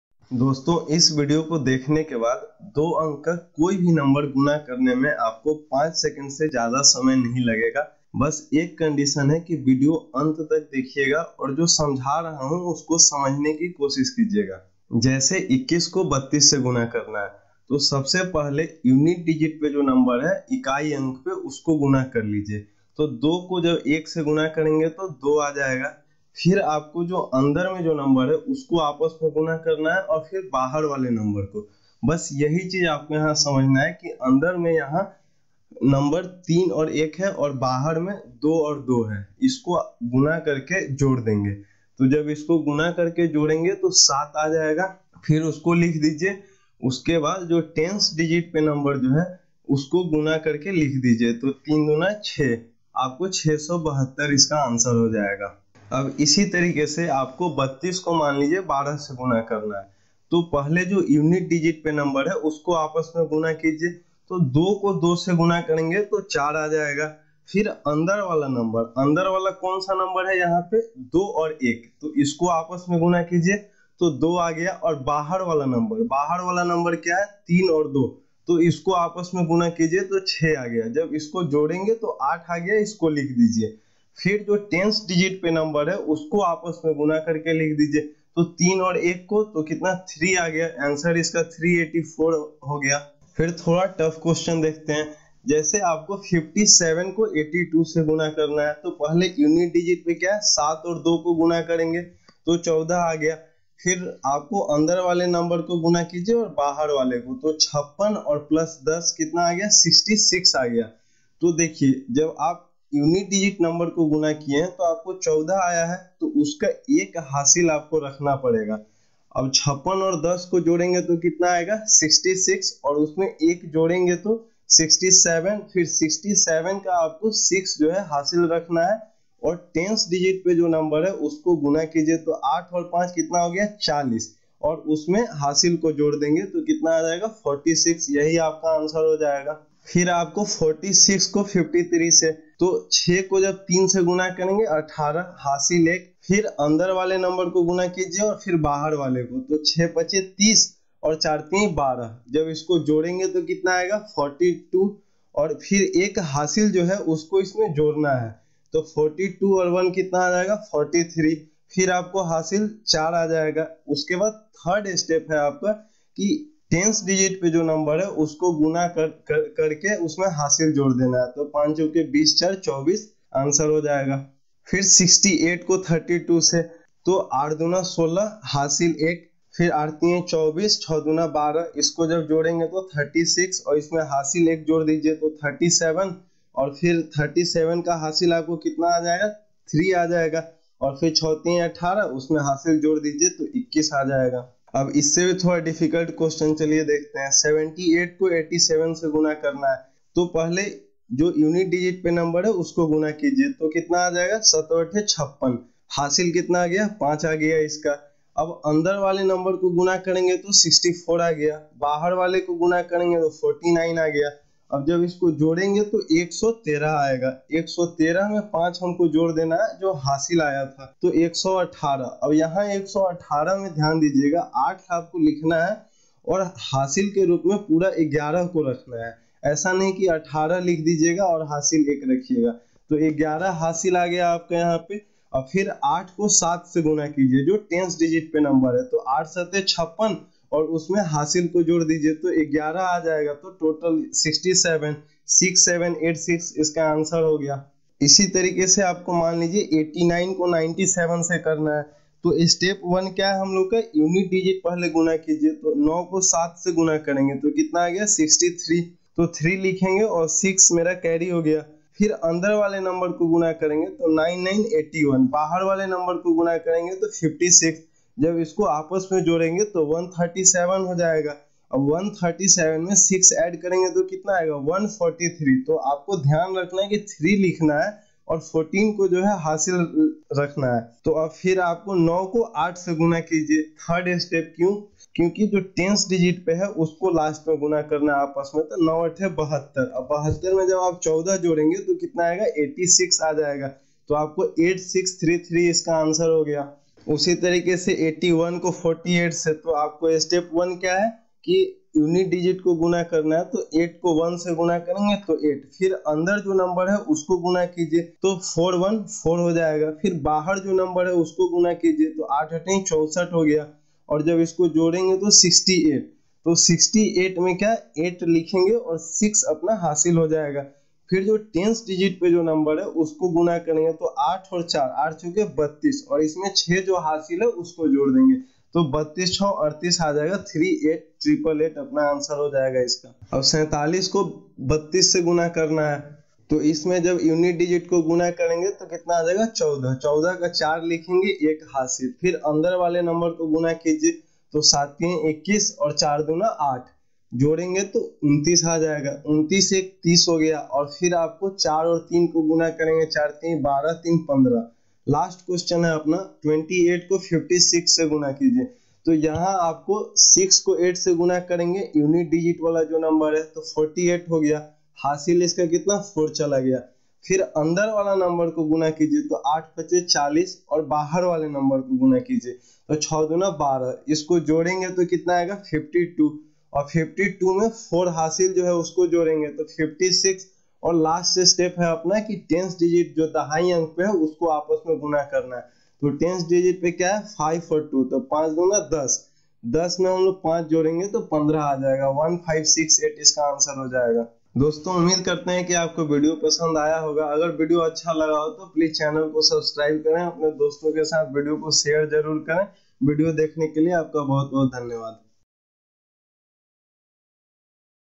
Multiply any two digit number in 5 sec| 5 सेकण्ड्स में गुना करें
Hello friends
In this video, we will learn how to multiply any two-digit number in less than 5 seconds. After watching the complete video you will multiply two-digit numbers faster than a calculator. This video covers the best trick to multiply a two-digit number.